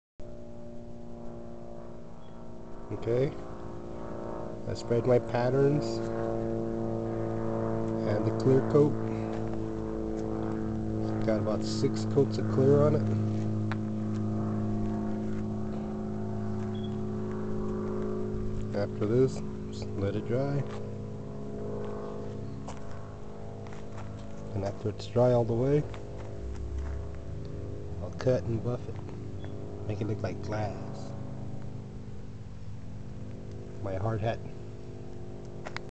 Part 2 of painting a hard hat
the patterns and clear coat are done on this hard hat